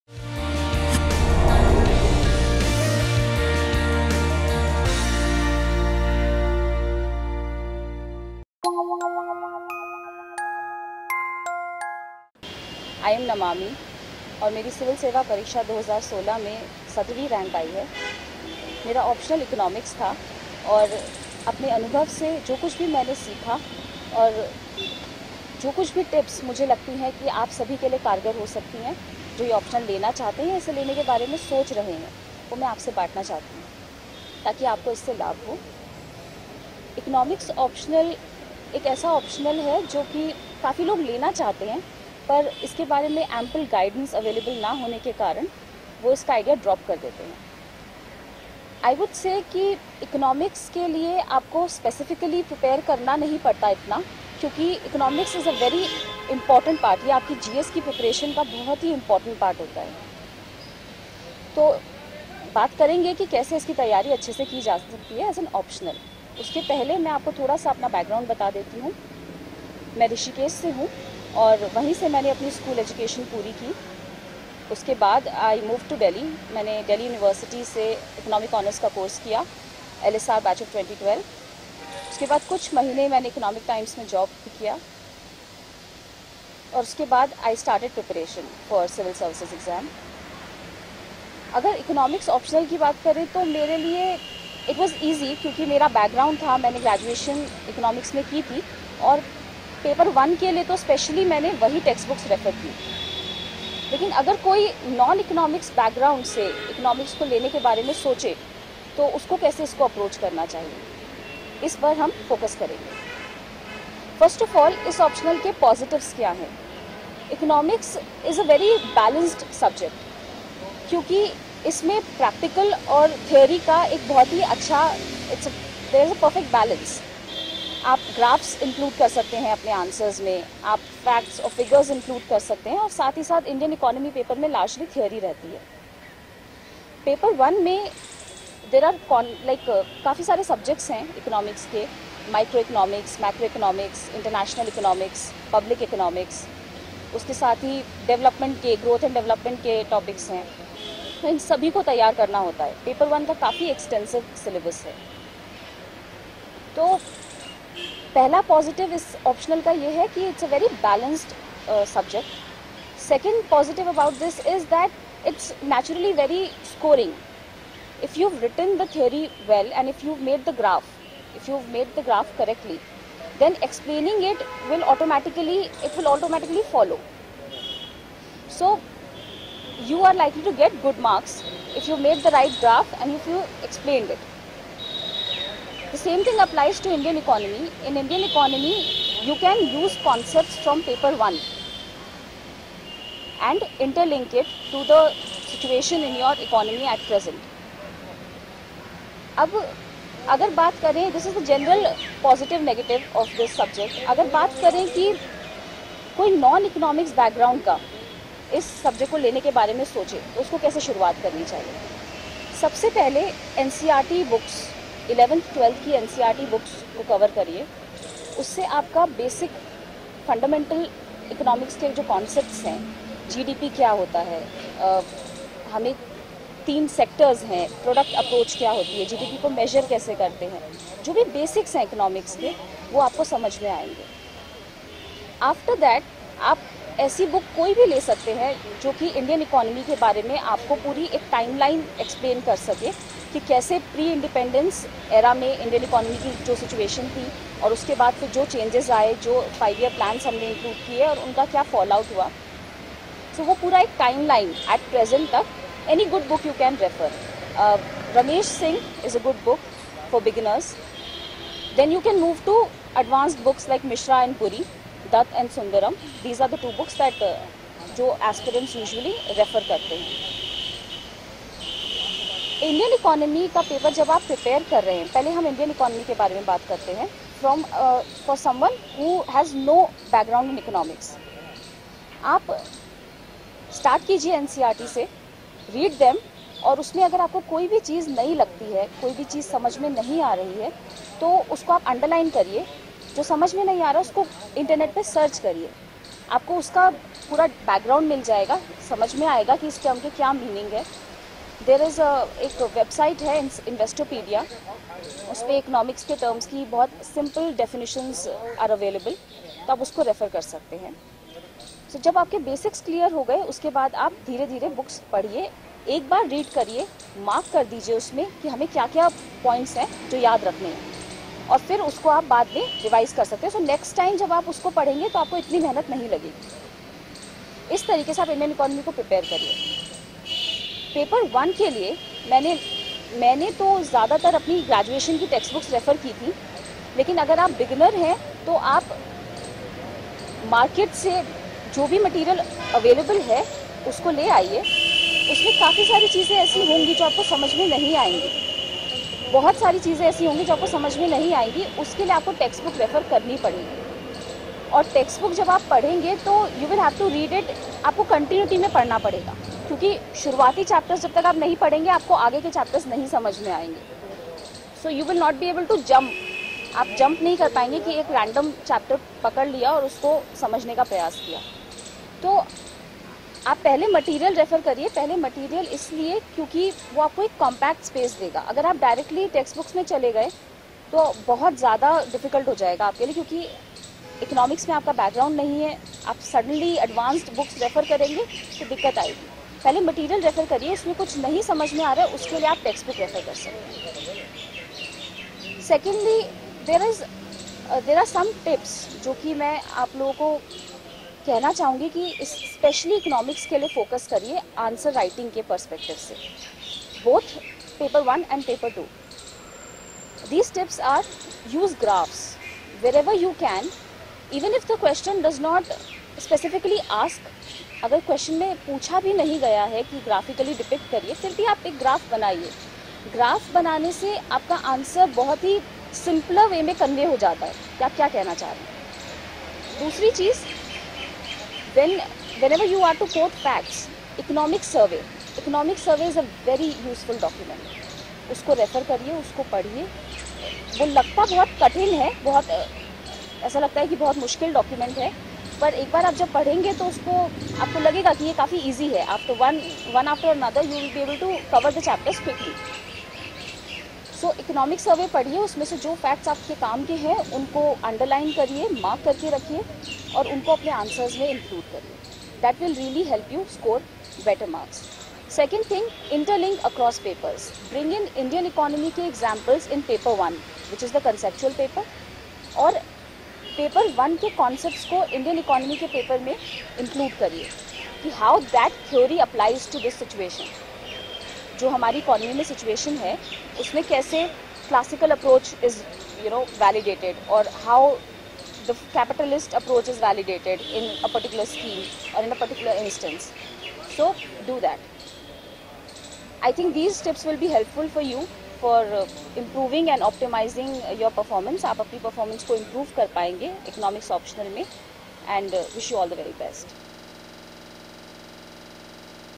CSE Optional | Economics | By AIR 17 | CSE 2016 Namami Bansal
Namami Bansal  (AIR-17 | CSE 2016) explains her strategy to crack Mains Exam with optional subject Economics. नमामि बंसल से जानिये कि सिविल सर्विस परीक्षा में वैकल्पिक विषय इकोनॉमिक्स की तैयारी कैसे करें.